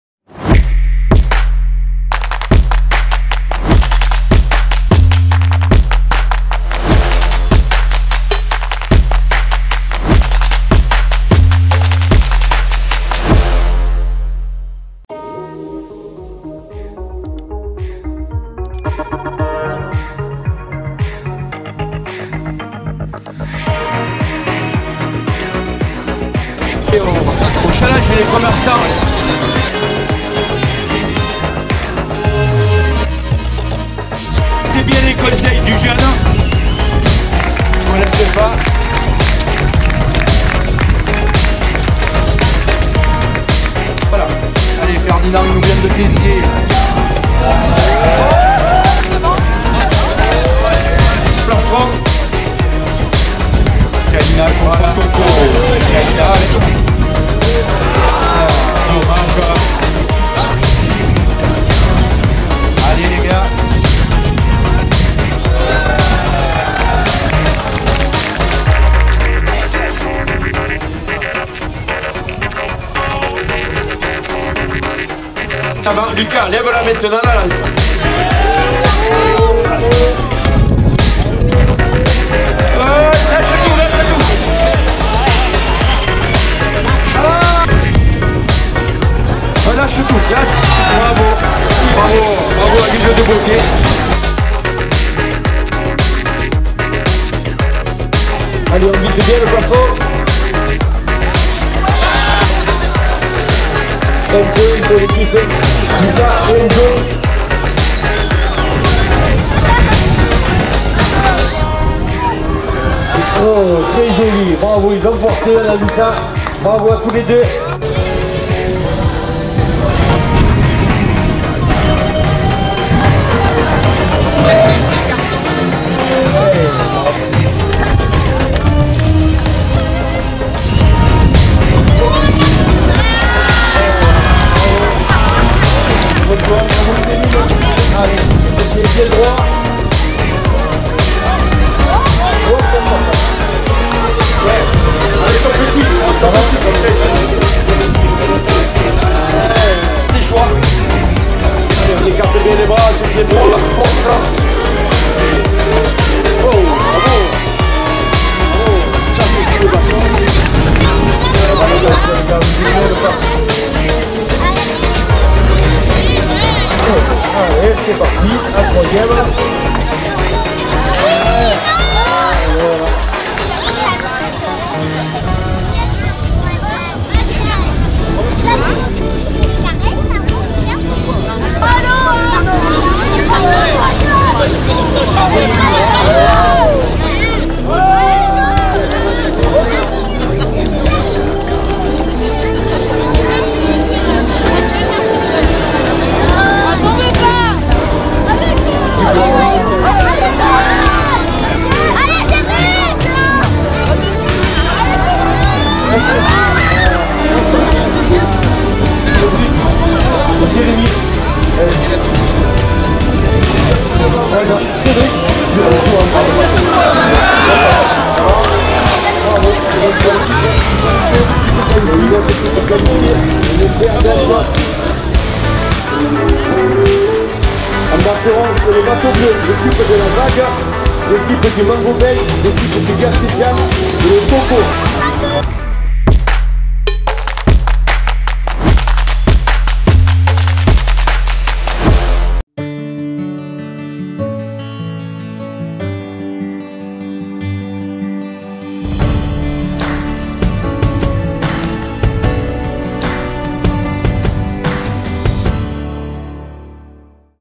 6E TOURNOI DE JOUTES PROVENÇALES du port a Hyères
Retour en Images Ce vendredi 21 juillet le 6E tournoi De joutes provençales sur le quai d'honneur du port hyères Ce vendredi 21 juillet avec jouteurs de Saint-Mandrier-sur-Mer Challenge des commerçants

Proposé par la ville d'Hyères et les commerçants du port